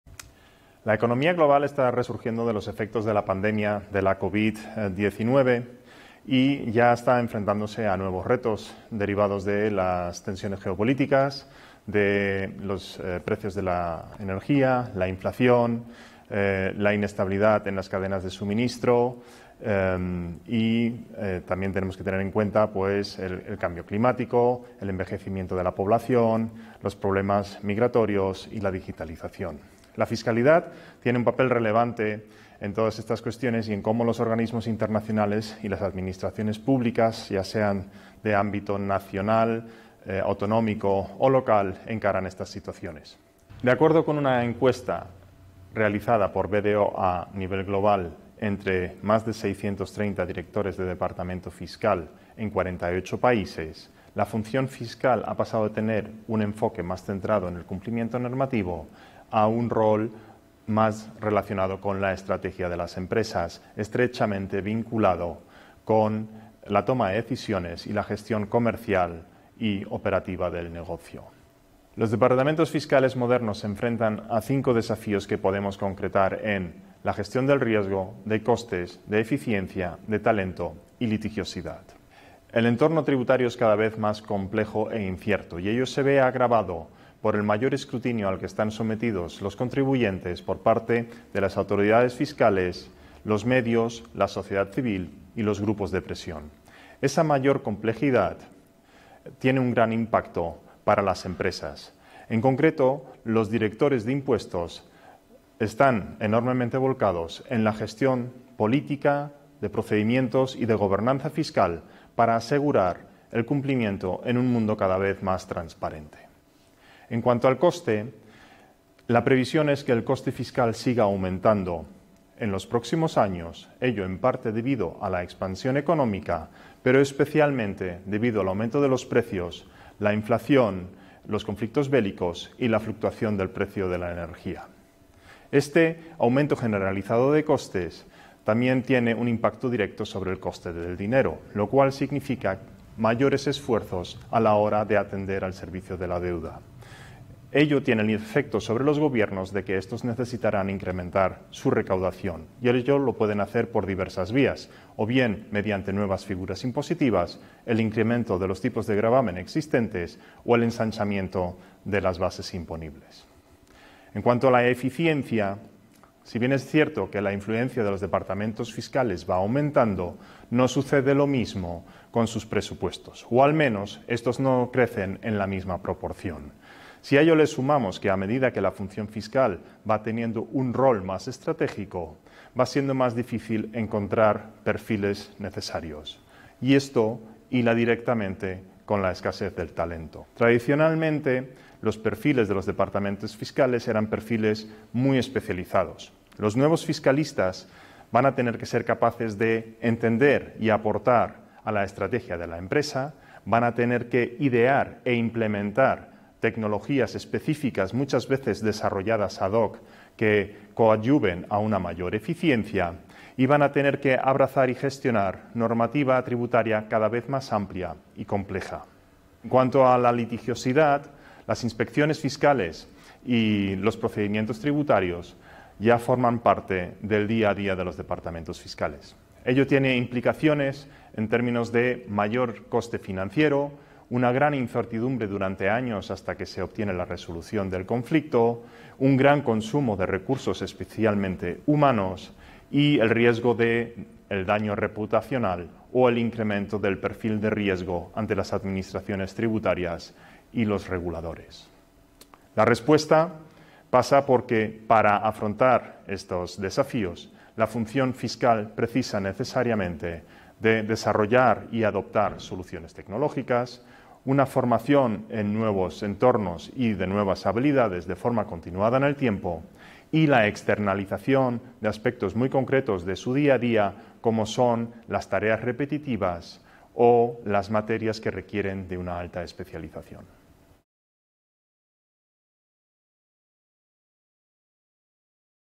Litigiosidad, riesgo, eficiencia… – Desafíos 23-24 -Función fiscal con Eugenio García
Con motivo del comienzo del nuevo curso académico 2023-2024, y con la ayuda de los profesionales de la comunidad Fide, analizamos los principales retos que preocupan hoy al mundo legal, empresarial, así como académico e institucional.

La economía global está resurgiendo de los efectos de la pandemia del COVID19. Se enfrenta a nuevos retos derivados de la tensión geopolítica, la inflación y muchos más factores. La fiscalidad tiene un papel relevante en todas estas cuestiones y en cómo los organismos internacionales y las administraciones públicas encaran estas situaciones.

Hoy nos reunimos con Eugenio García, socio de BDO España, que nos indica los retos que tiene que afrontar la función fiscal en las empresas:

1. Riesgo: Entorno tributario cada vez más complejo e incierto. Escrutinio por parte de autoridades fiscales, los medios, la sociedad civil y grupos de presión.

2. Coste o carga fiscal creciente como consecuencia de la pandemia, el cambio climático y otros desafíos demográficos a nivel global.

3. Eficiencia: Existencia de una necesidad creciente para reducir gastos, a la vez que aumenta la carga administrativa. Los departamentos fiscales deben producir más con menos recursos, además de implementar procesos más eficientes.

4. Falta de talento: La función fiscal es transversal. Ello hace que los equipos tienen que disponer de un amplio abanico de funciones para entender y aplicar correctamente la nueva normativa tributaria cada vez más numerosa y diferente, además de implementar los avances tecnológicos y tener una visión global de la empresa.

5. Litigiosidad: En un mundo de tensiones geoeconómicas, mayor complejidad y mayores riesgos fiscales, la litigiosidad se convierte en una realidad habitual para las empresas

Continuamos estudiando los principales desafíos de esta y otras muchas áreas relevantes. Podéis encontrarlos todos en nuestra web.

Os animamos a compartir esta información y continuar con el debate.